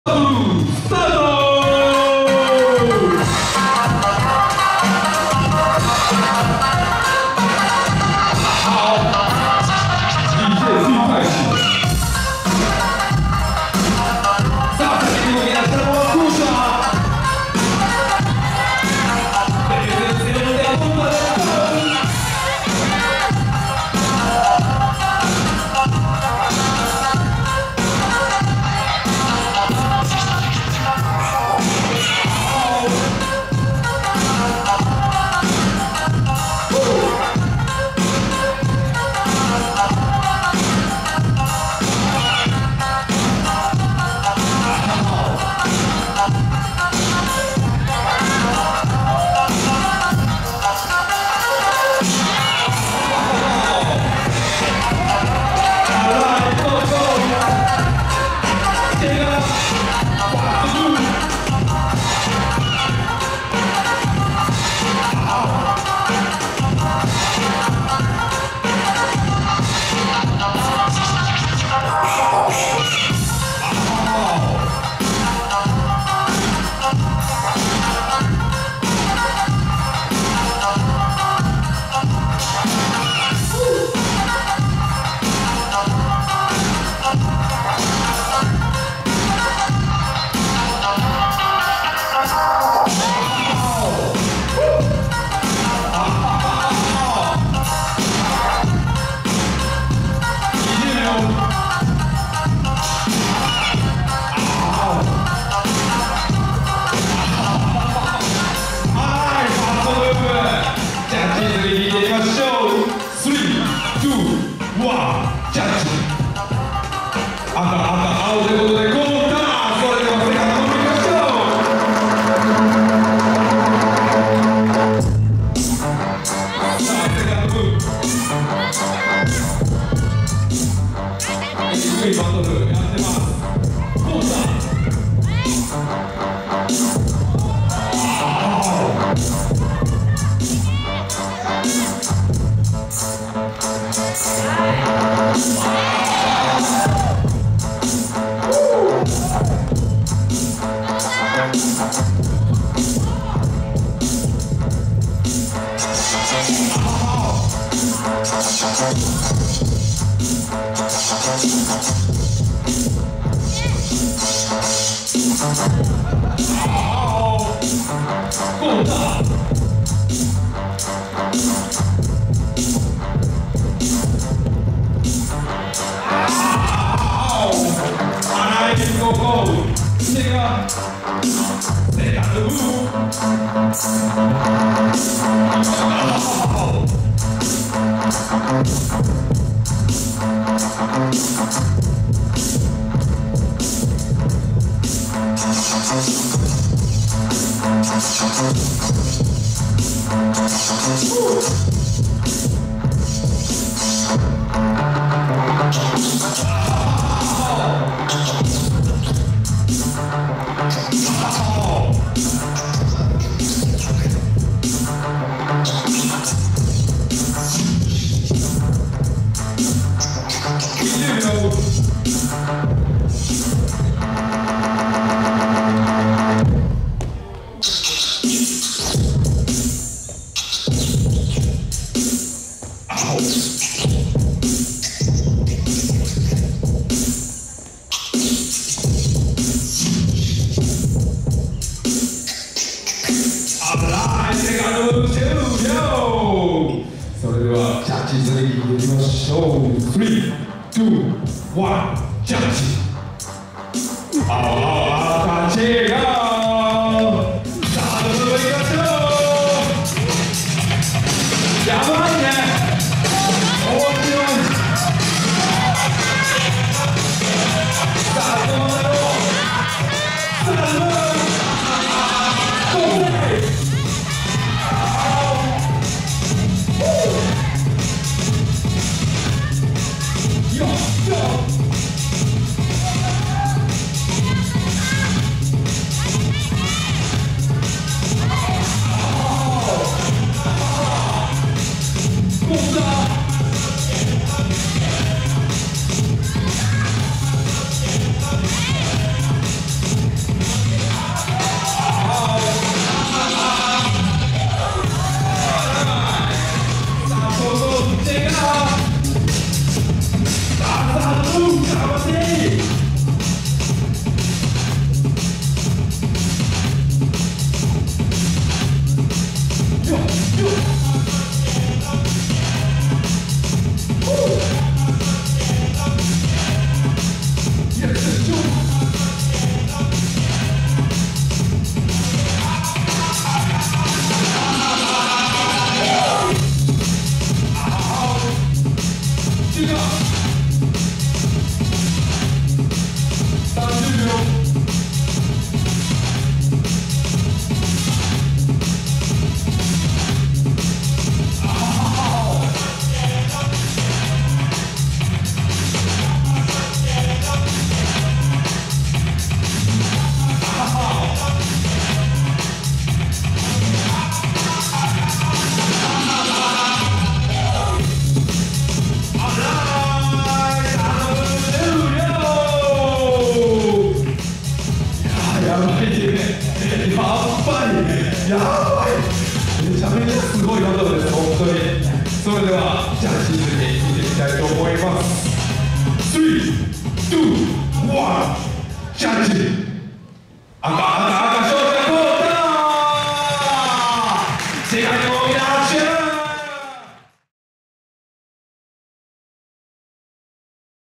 【 Kohta vs CHEGA 】DOUBLE DUTCH ONE'S 2023-2024 SCENE3 MEN'S BEST8
DOUBLE DUTCH ONE'S 2023_2024 SCENE3
MEN’S SECTION BEST8

[WINNER]
Kohta

[ MC ]
ANDERSON

[ DJ ]
t.taishi

[ JUDGE ]
TATSUYA
Kozy a.k.a. JUNKNESS
KEITA NAKAMURA

[ TURNER ]
MAXIMA
HA-CHAN